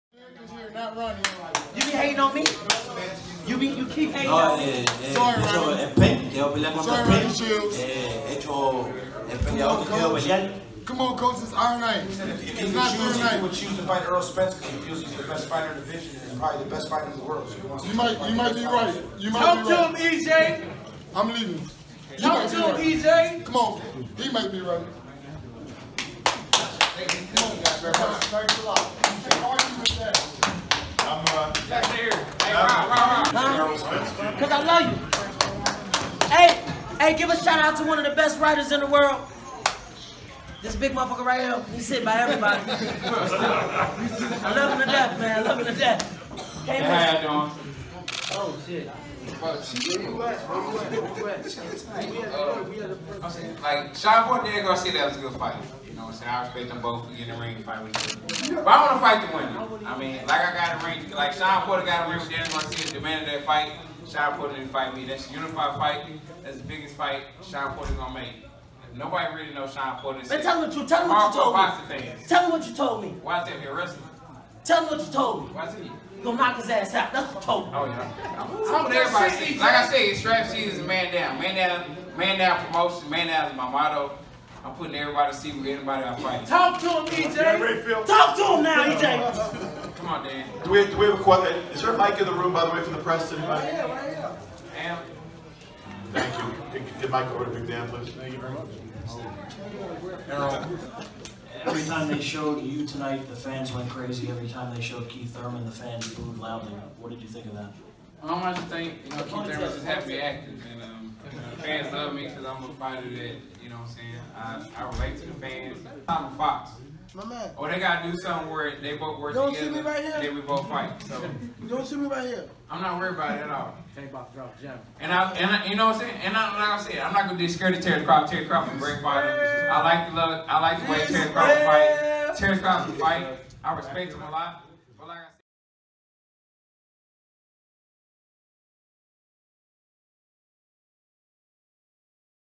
ADRIEN BRONER AND GERVONTA DAVIS STRAIGHT CLOWNIN; WILIN' OUT AS ERROL SPENCE SPEAKS TO MEDIA
FightHype.com was on hand at the Barclays Center in Brooklyn, New York where Shawn Porter defeated former world champion Danny Garcia via unanimous decision to capture the WBC title. Check out the scene immediately after the fight as those in attendance reacted to the performance.